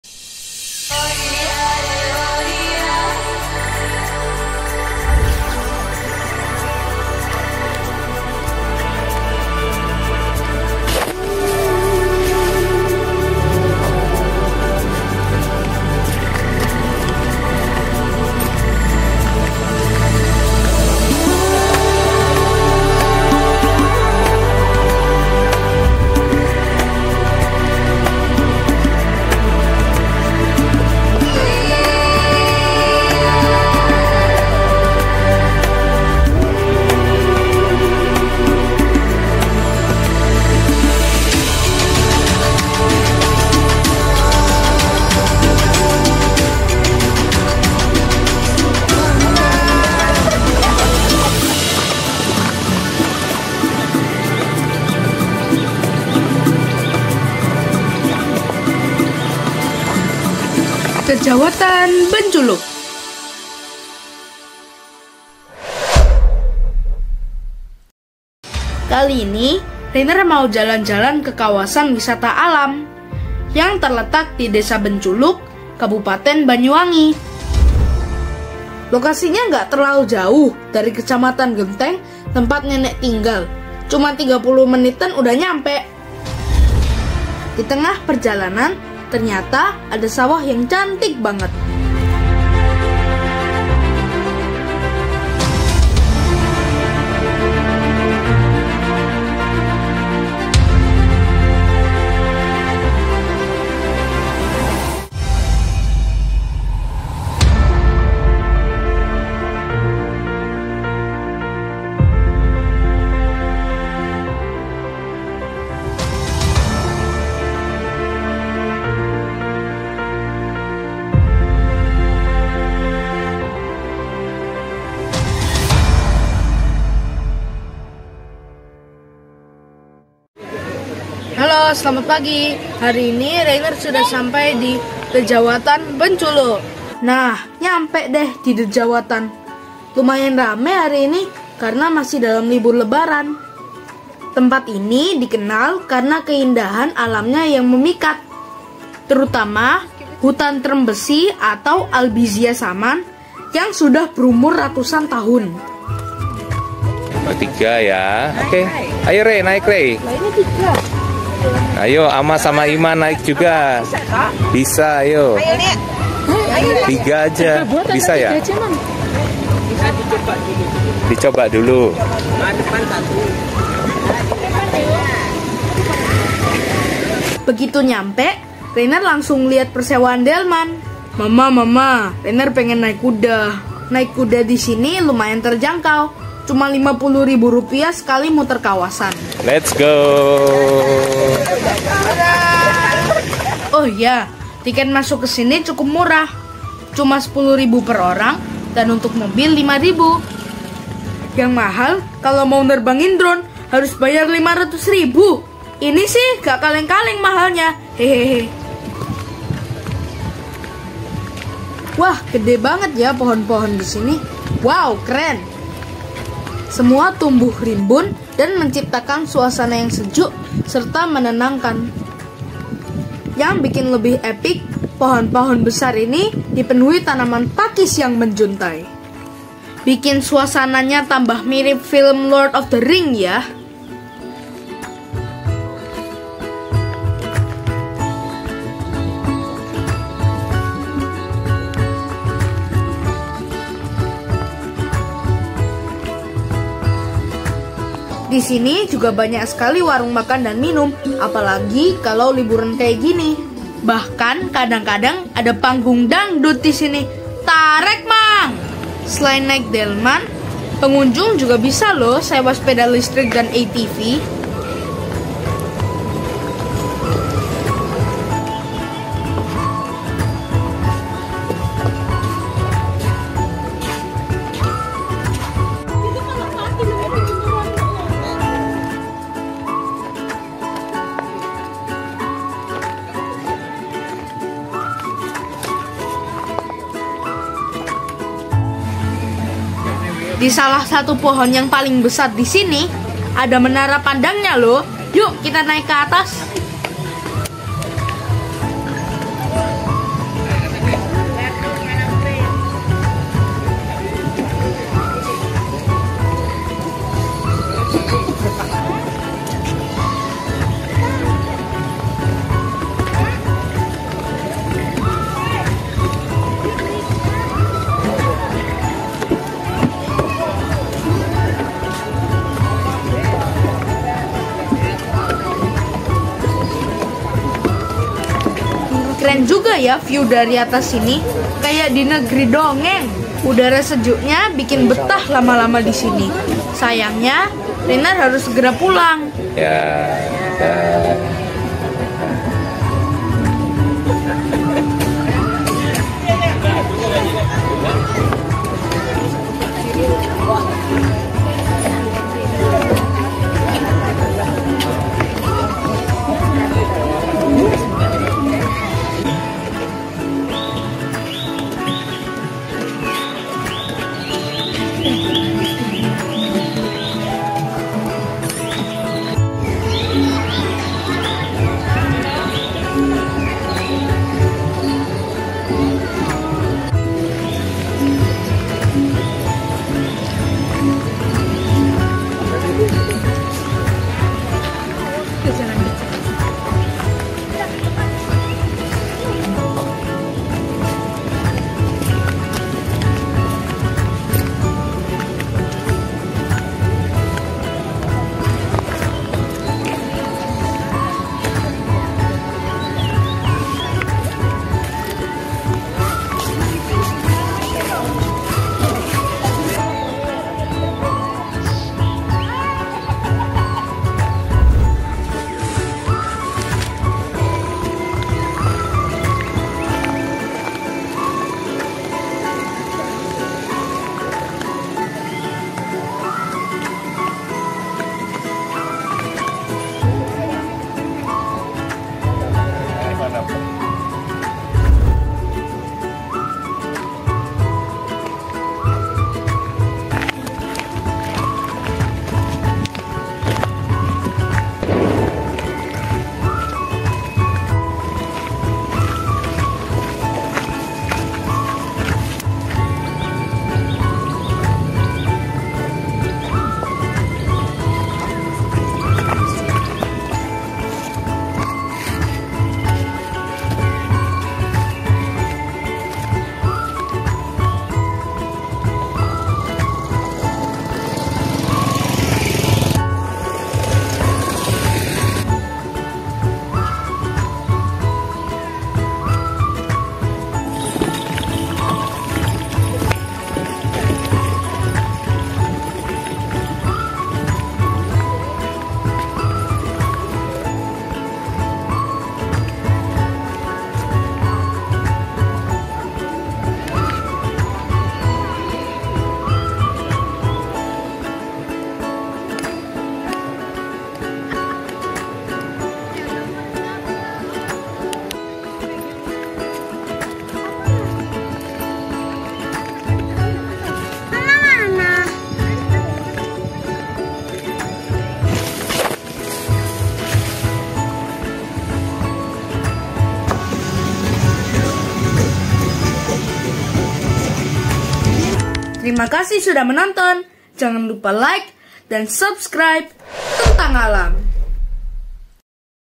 Hutan Exotis De-Djawatan Benculuk saat LiburLebaran 3
De Djawatan terletak di Desa Benculuk, Kecamatan Cluring, Banyuwangi

Daya tarik tempat ini adalah pohon2 trembesi nan exotis yang usianya sudah ratusan tahun.
Pengunjung yang ingin menikmati kawasan ini cukup membayar tiket masuk Rp 10.000 per orang. Parkir mobil Rp. 5.000.
Jam Buka De Djawatan mulai pukul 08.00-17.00 WIB.